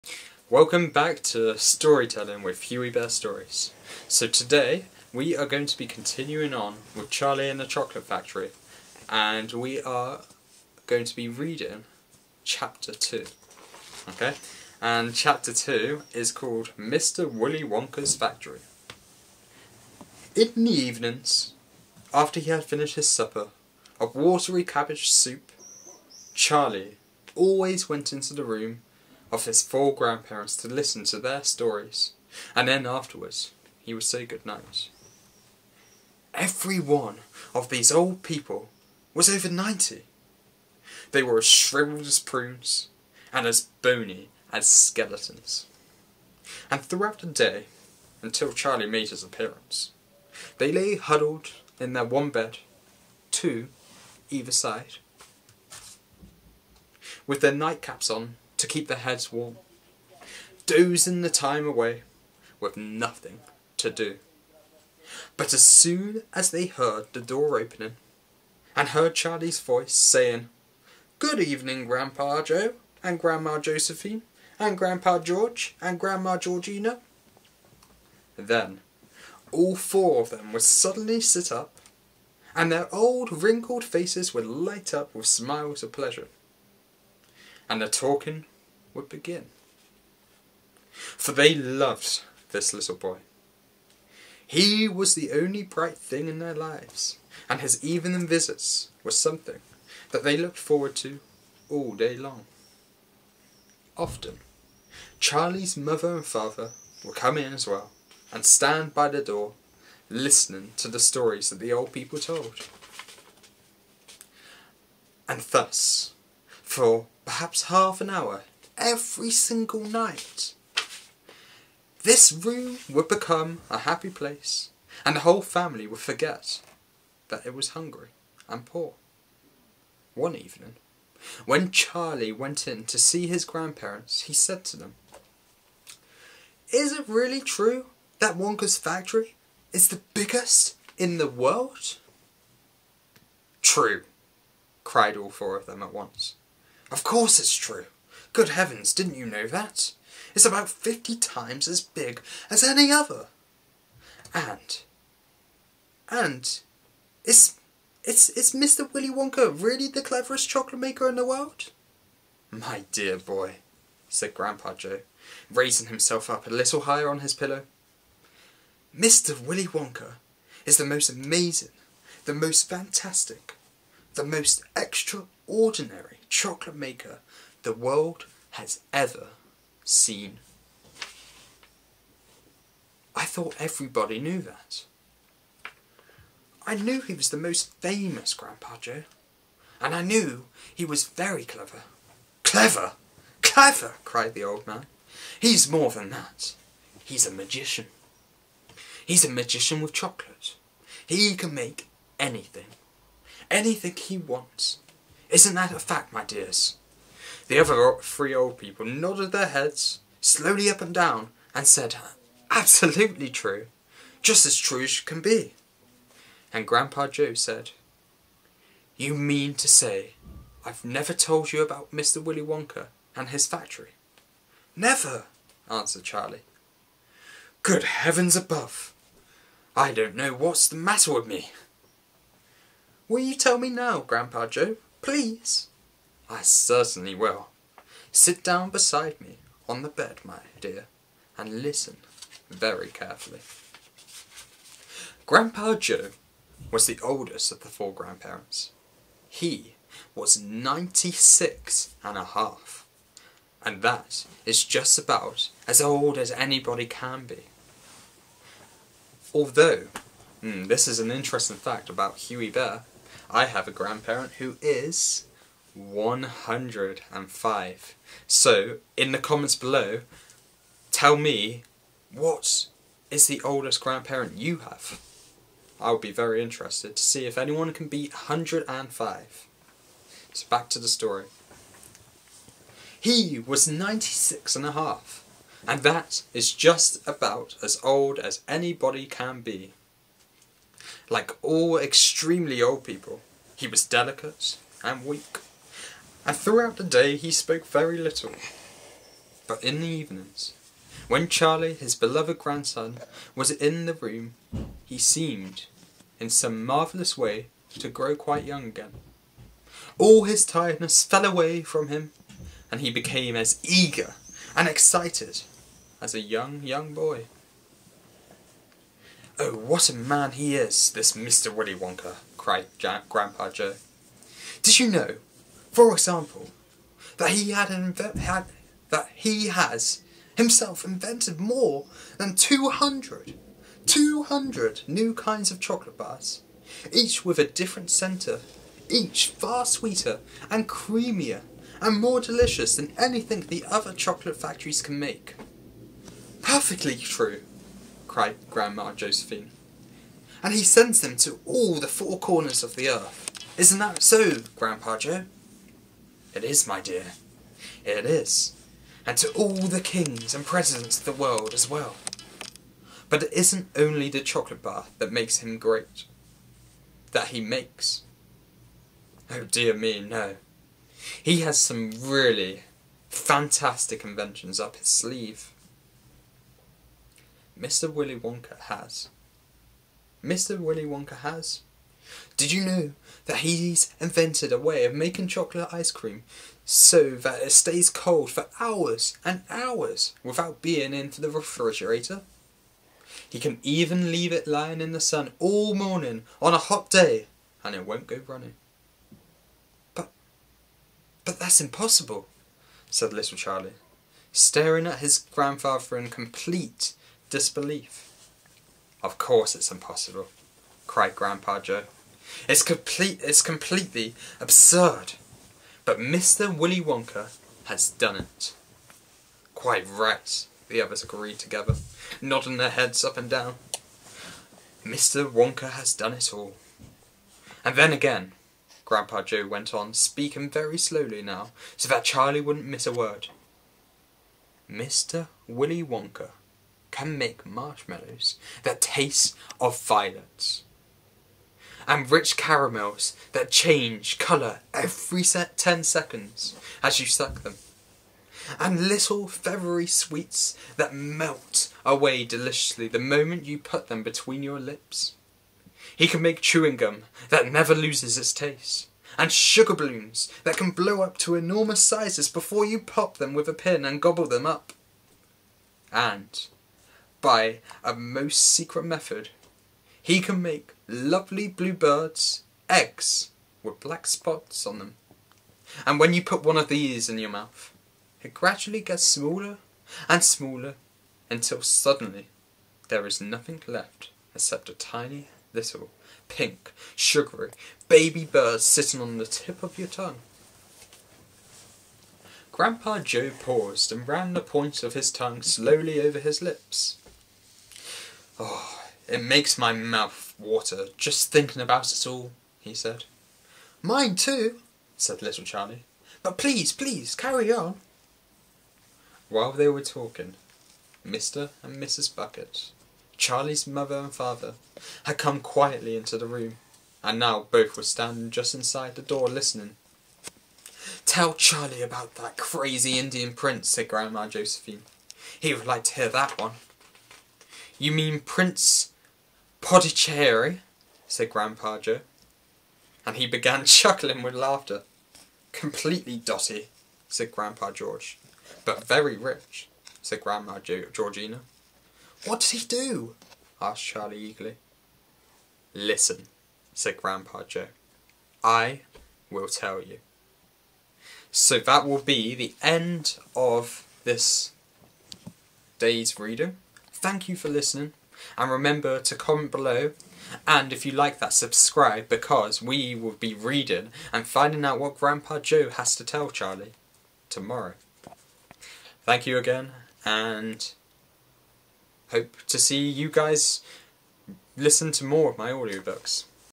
Charlie and The Chocolate Factory Bedtime Audiostory For Kids Chapter 2
Welcome back to storytelling with Huwey Bear Stories. Today I will be reading the second chapter of Charlie and The Chocolate Factory by Roald Dahl.